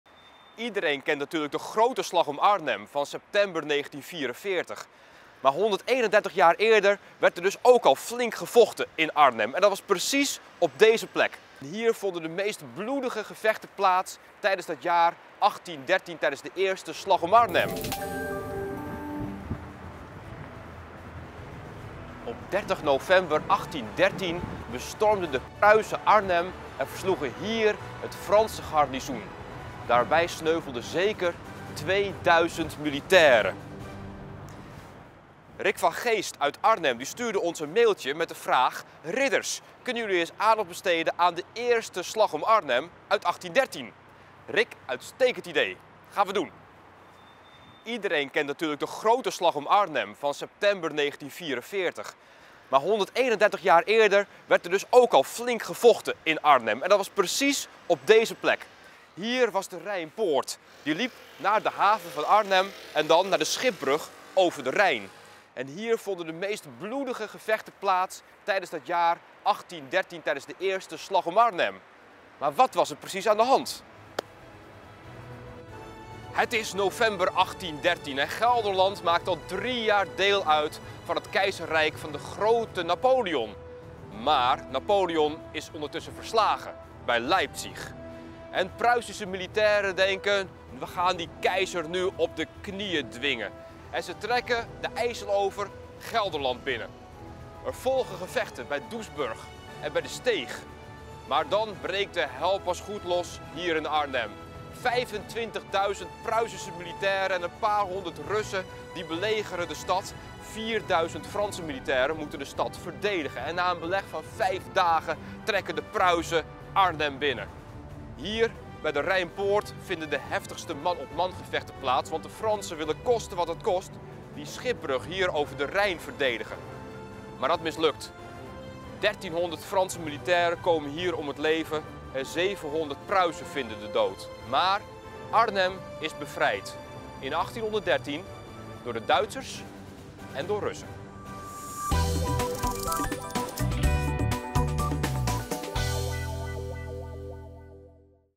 Duitsers en Fransen vochten om Arnhem | Ridders van Gelre
Heb jij dat ook? Dat je bij sommige historische bouwsels, hekken, beelden, palen, gevelstenen en gebouwen in je omgeving denkt: "Wat is dat eigenlijk, hoe oud is het en waar diende het voor?" De Ridders van Gelre zoeken het voor je uit!

Iedereen kent de Slag om Arnhem in september 1944, maar wist jij dat er 131 jaar vóór Operatie Market Garden al een andere Slag om Arnhem heeft plaatsgevonden? Pruisen en Russen probeerden de stad te veroveren op de Fransen. Hoe dat zat, zie je hier!